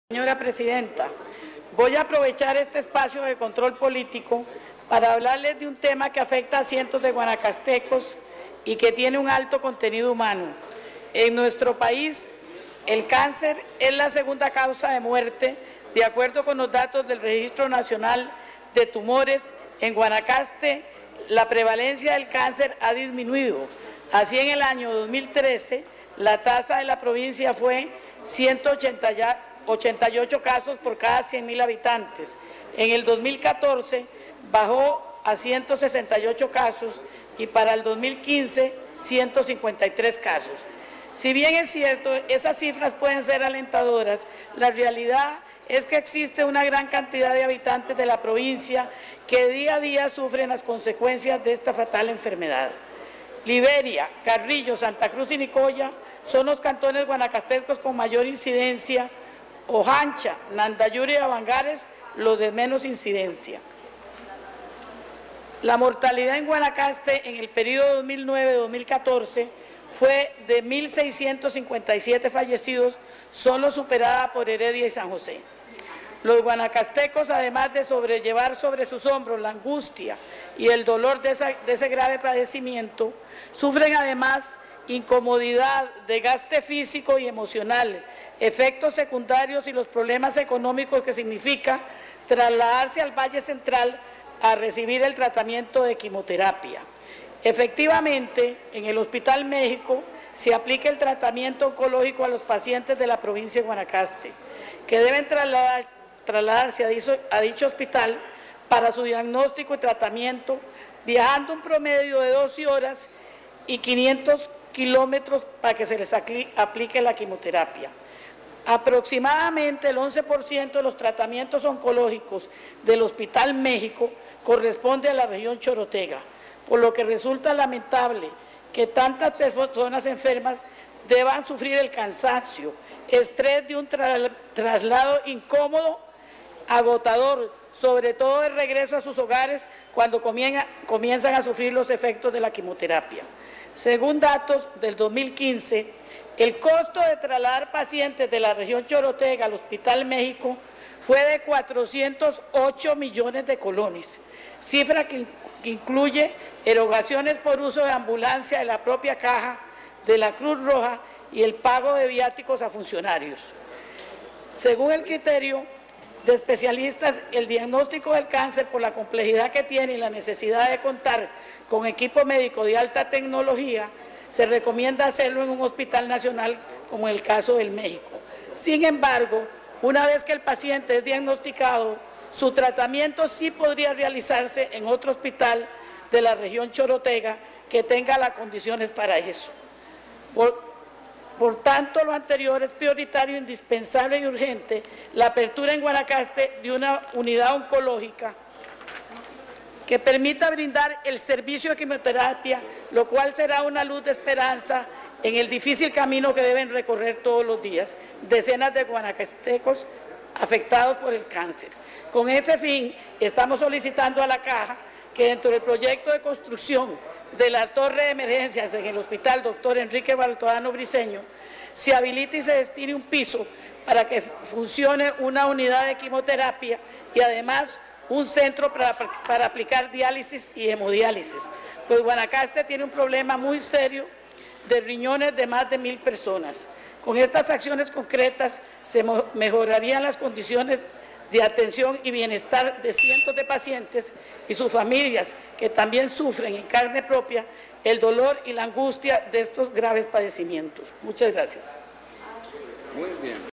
Aida Montiel “Guanacaste necesita una unidad oncológica”
“La mortalidad en Guanacaste en el periodo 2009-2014 fue de 1659 fallecidos, sólo superada por Heredia y San José.

Los guanacastecos y guanacastecas, además de sobrellevar sobre sus hombros la angustia y el dolor de ese grave  padecimiento, sufren además incomodidad, desgaste físico y emocional, efectos secundarios y  los problemas económicos que significa trasladarse al Valle Central a recibir tratamiento de quimioterapia”